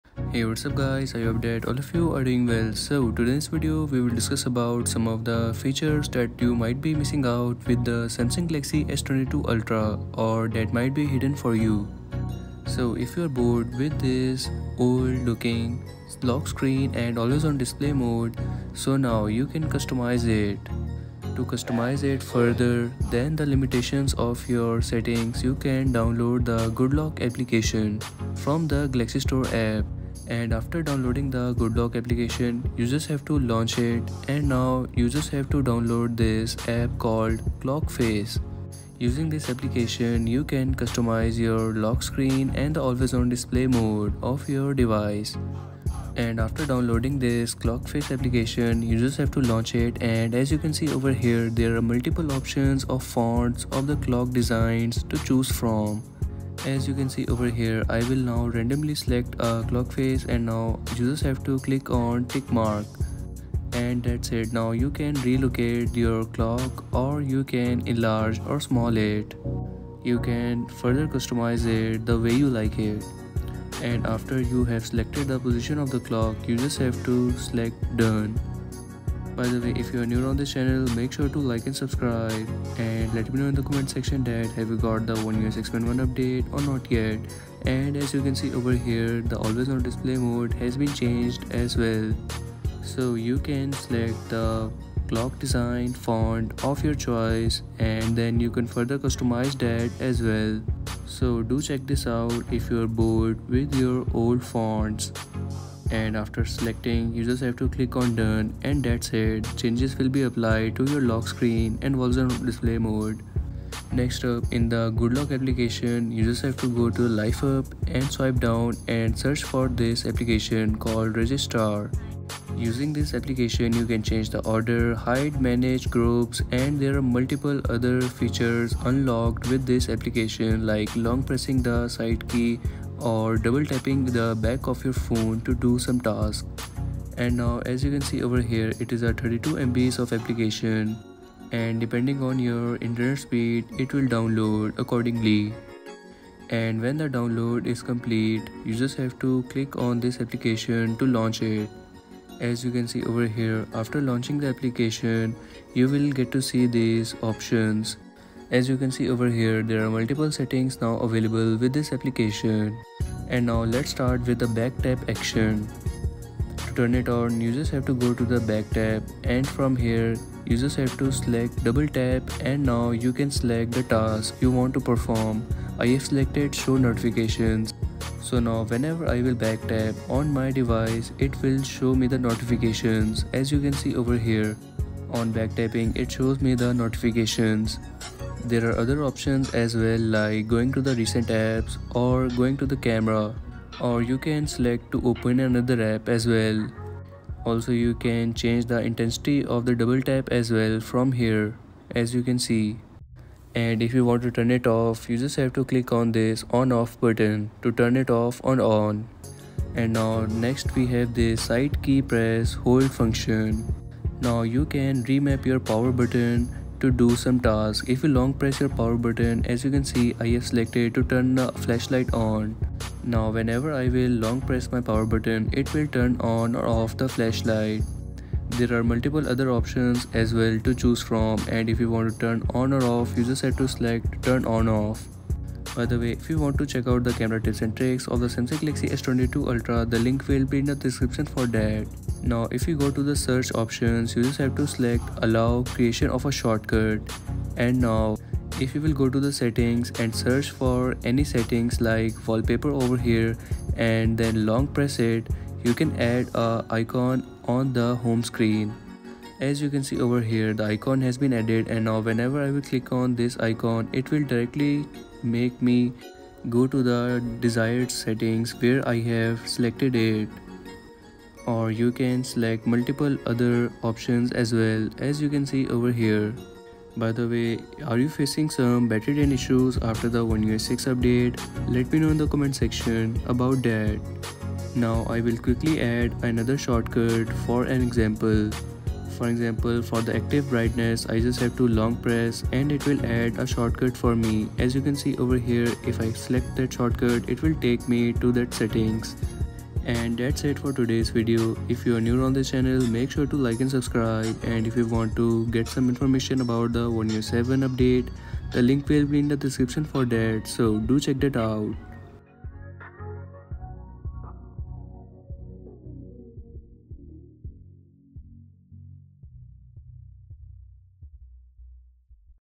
Samsung One Ui 6.1 - Top Powerful Hidden Features! Must Try
So these were some of the hidden features that you should try out! With the Good Lock app.